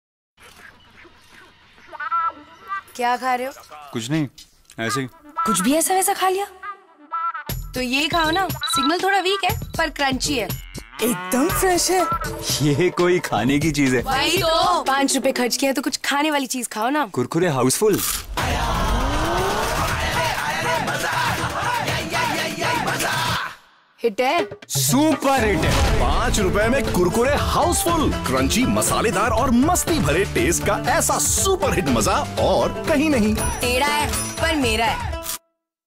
Parineeti Chopra in Kurkure New 5 Rs Pack
Kurkure New 5 rs pack ad - ye bhi koi khane ki cheez hai kurkure khao 5 Rs mai